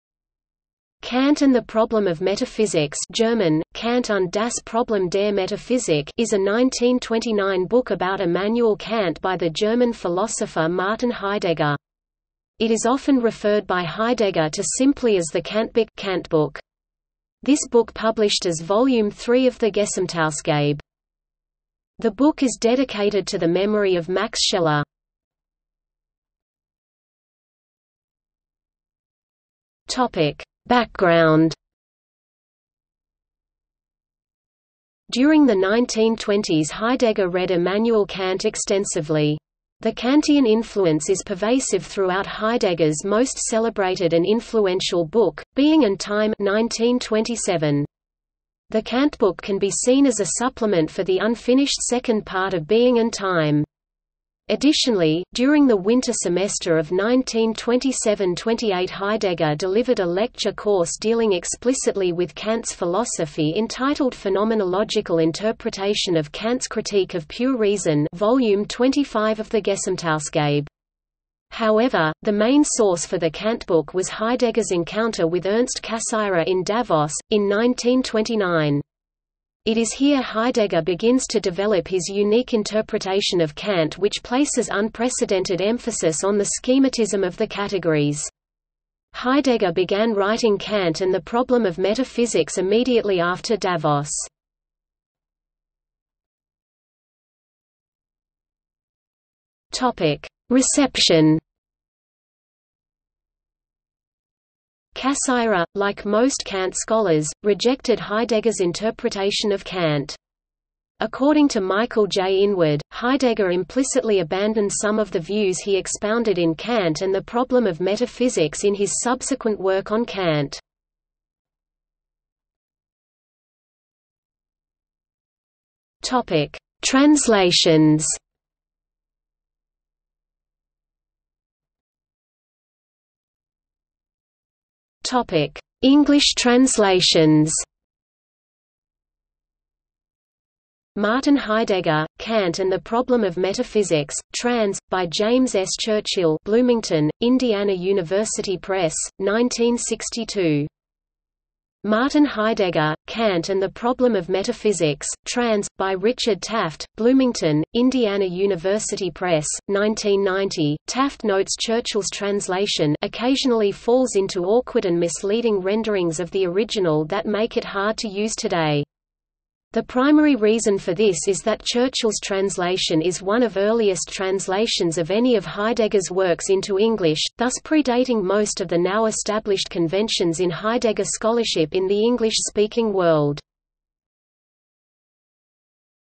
Kant und das Problem der Metaphysik | Wikipedia audio article
00:00:26 1 Background
00:01:36 2 Reception
00:02:02 3 Translations
00:02:11 3.1 English translations

- Socrates

SUMMARY
Kant and the Problem of Metaphysics (German: Kant und das Problem der Metaphysik) is a 1929 book about Immanuel Kant by the German philosopher Martin Heidegger. It is often referred by Heidegger to simply as the Kantbuch (Kantbook). This book published as volume 3 of the Gesamtausgabe.
The book is dedicated to the memory of Max Scheler.